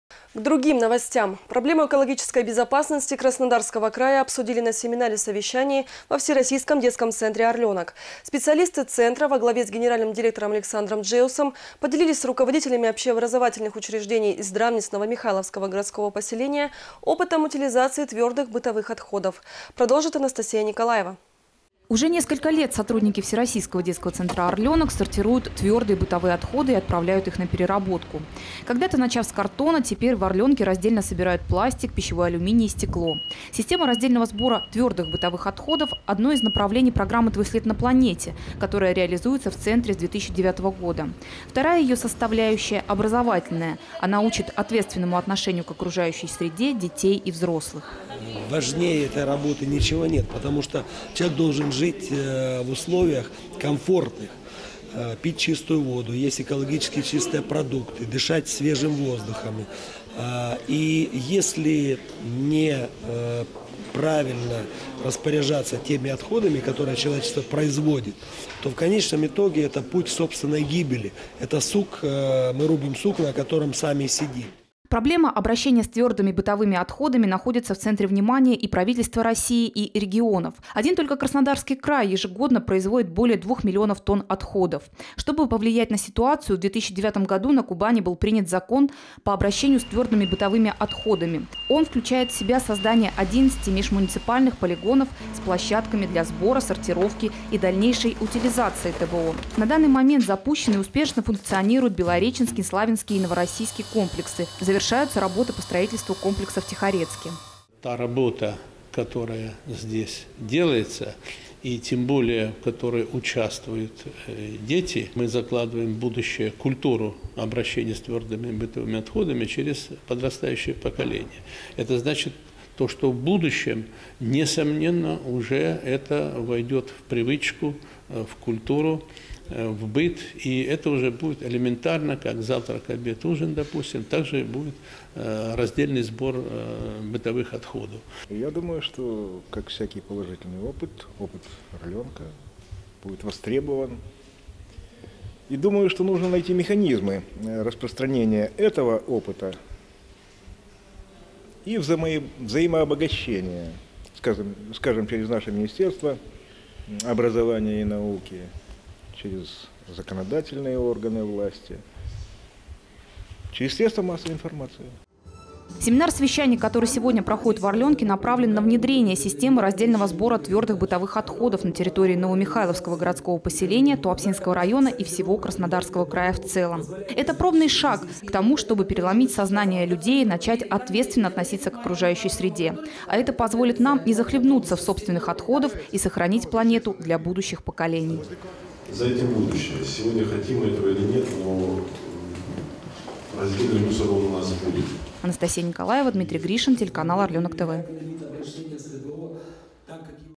Совещание «Проблема экологической безопасности»
Специалисты Орленка поделились с руководителями общеобразовательных учреждений Новомихайловского городского поселения опытом утилизации твердых бытовых отходов.
Корреспондент: Анастасия  Николаева
Оператор: Дмитрий Гришин
Монтаж: Дмитрий Гришин